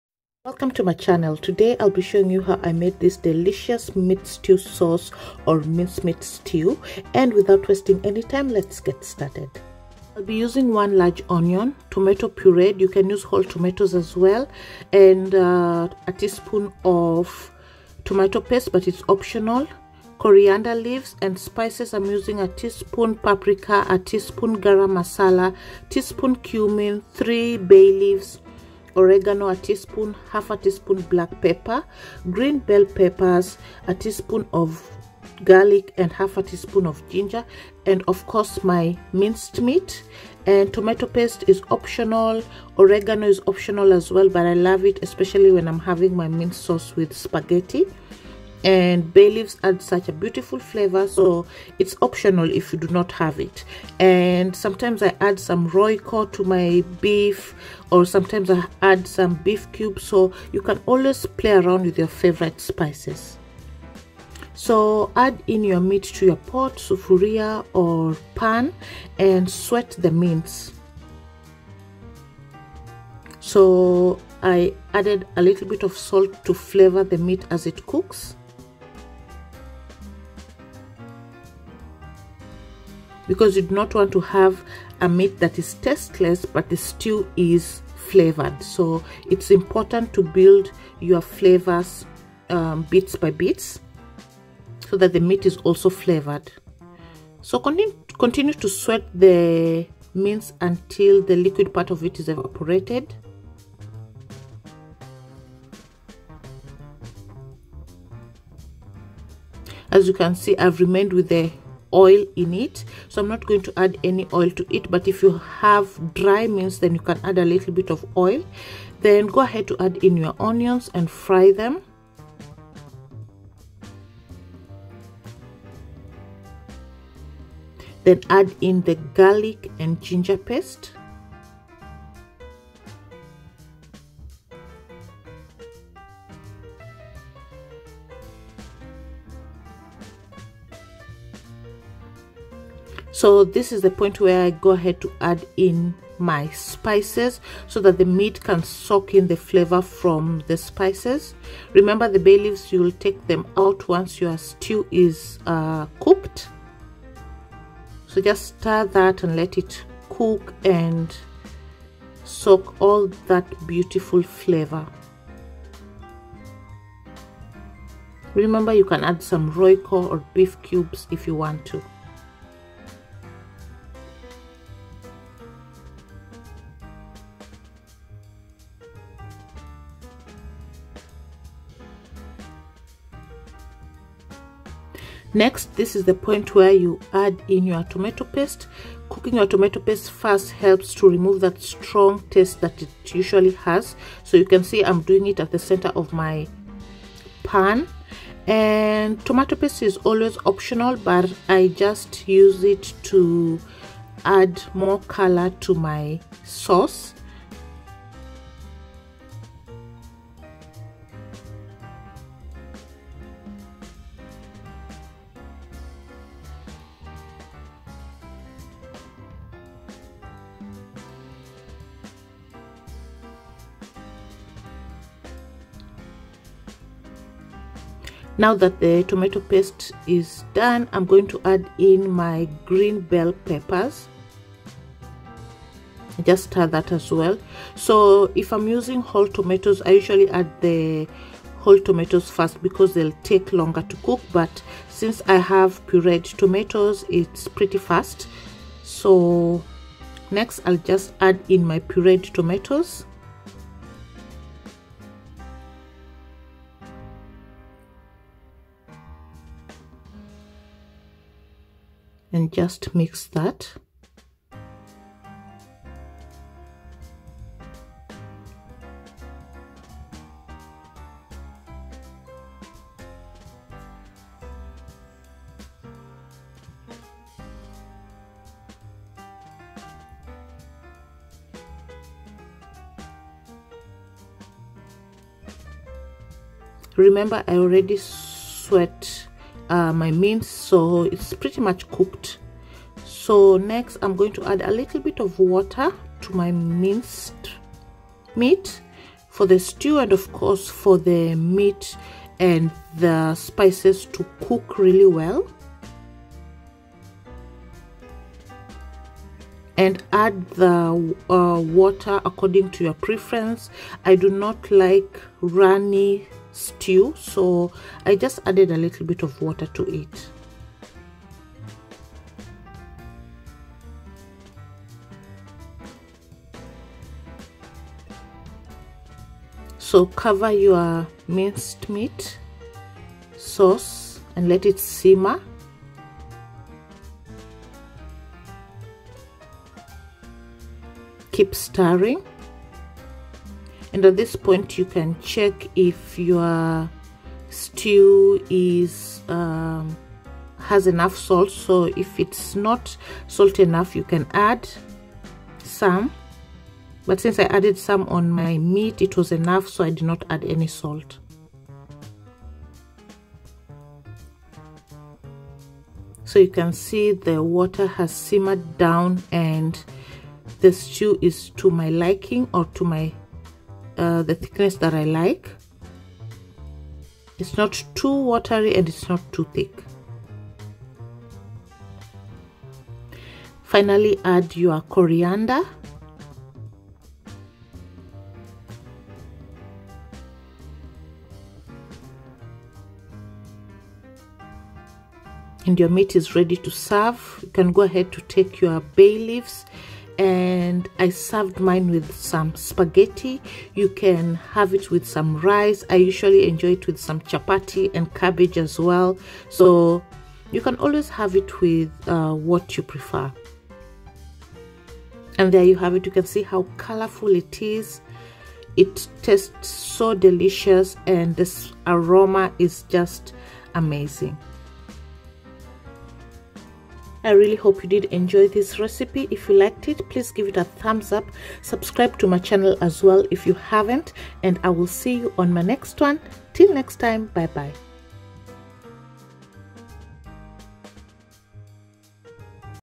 How to make Minced Meat Stew || Minced Beef Stew Recipe.|| Minced Sauce
Minced Meat is delicious Stew that you can enjoy with Spaghetti, Rice , Chapati etc
INGREDIENTS:
700g Ground Beef
1 Large Onion
1 Tsp  Garlic
1/2 Ginger
Salt to taste to your preference
1/2 tsp black pepper
1 tsp paprika
1 Tbsp Tomatoe paste
1 Green bell pepper
1 bunch Corriander
1 Tsp Cumin
1 Tsp Gharama Masala
Royco (optional)
fresh corriander
Tomatoe puree or 5 ripeTomatoes
Tomatoe Paste 1 Tbsp